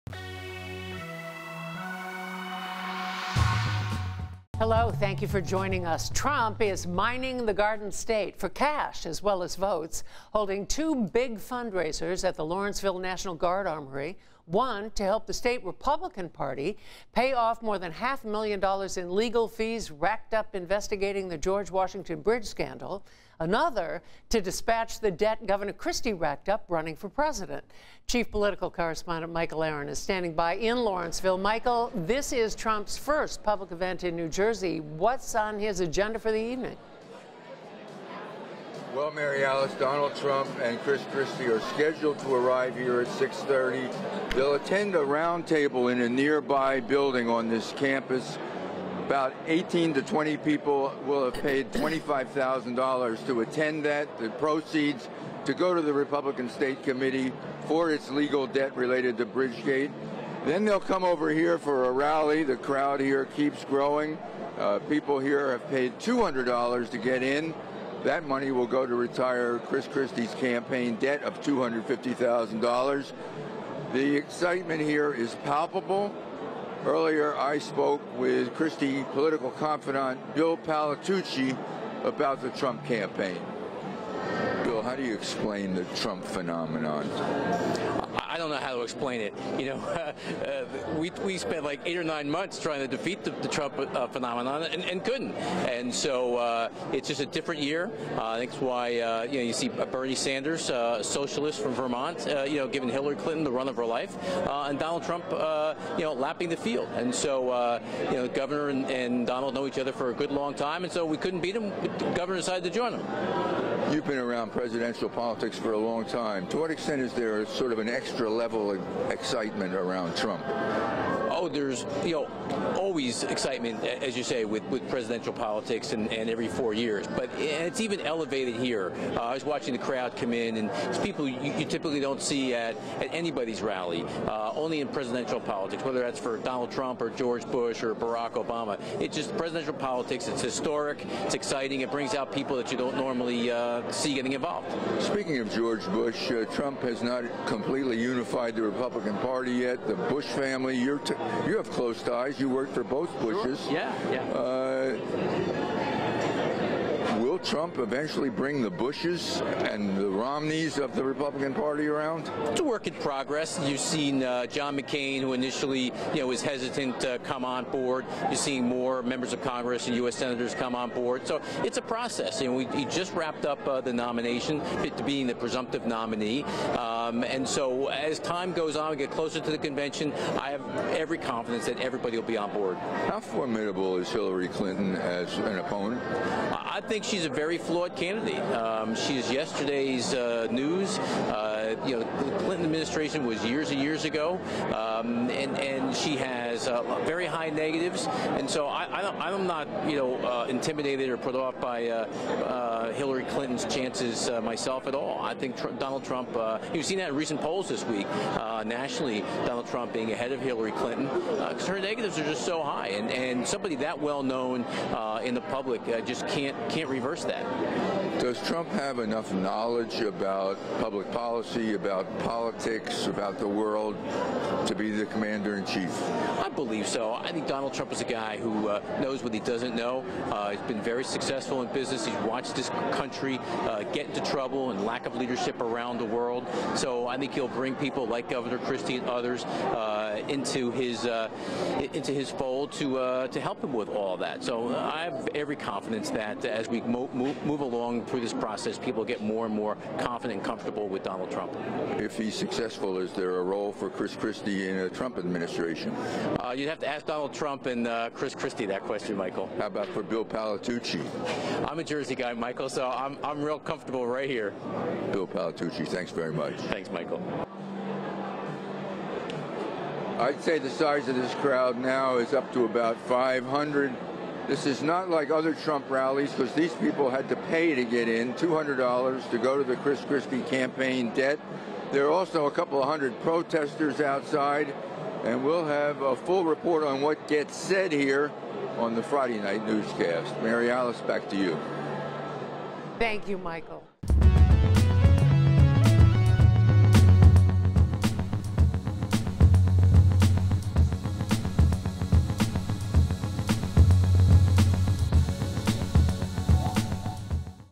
Donald Trump Holds New Jersey Rally, Fundraisers
Presidential hopeful Donald Trump makes his first campaign visit to the Garden State with two fundraisers and a rally in Lawrenceville with Gov. Christie.